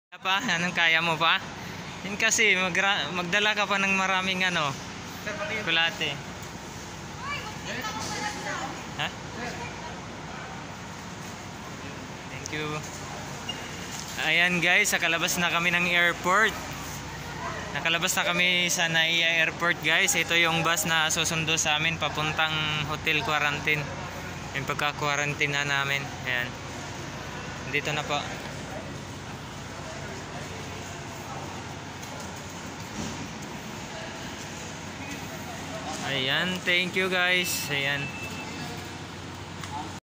Bus na nakaabang sa labas ng Airport para sa aming mga OFW papuntang hotelquarantineFacilities.
Bus na nakaabang sa labas ng ninoy aquino international Airport para sa aming mga ofw papunta sa kanya-kanya naming mga hotelquarantineFacilities.